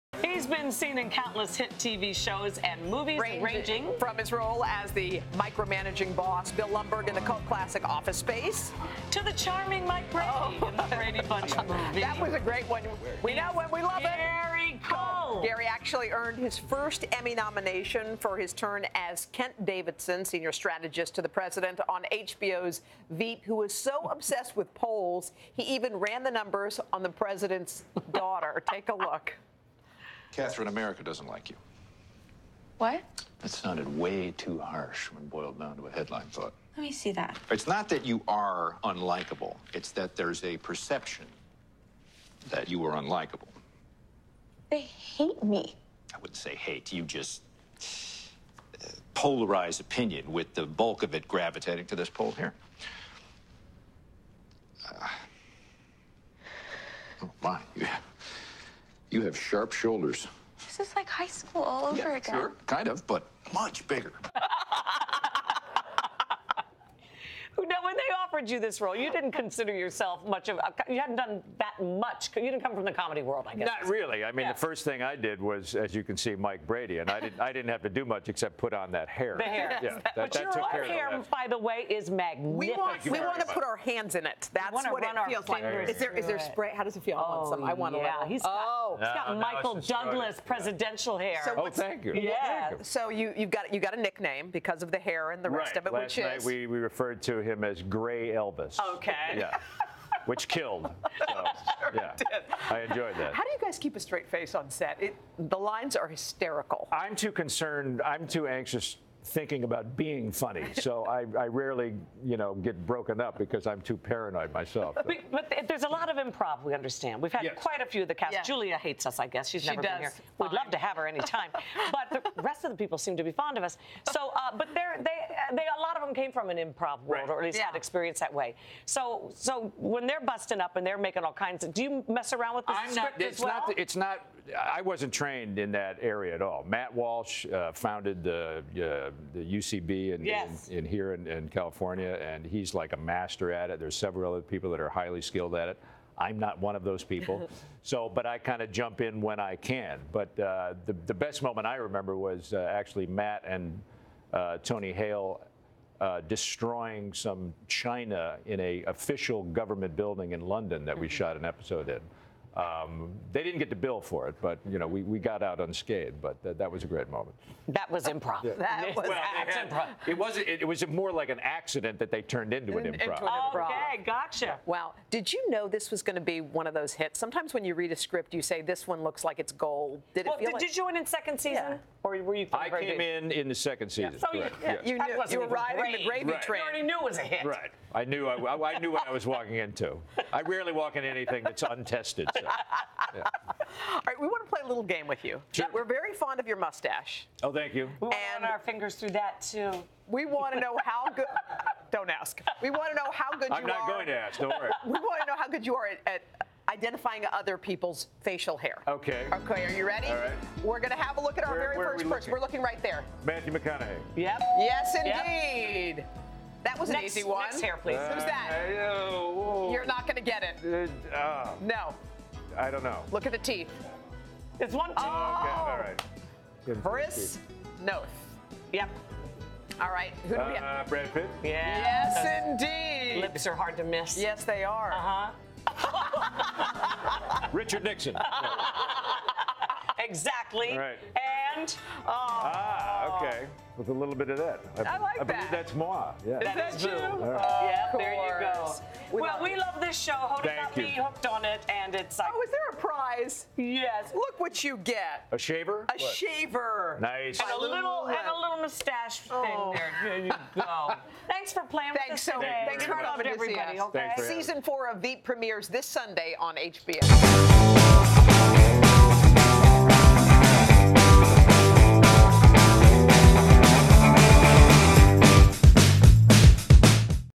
Gary Cole Was Anxious On ‘Veep’ | TODAY
Actor Gary Cole plays the senior strategist to the president on the HBO show “Veep,” and he tells Kathie Lee and Hoda that he’s often anxious on set, hoping his lines come across as funny.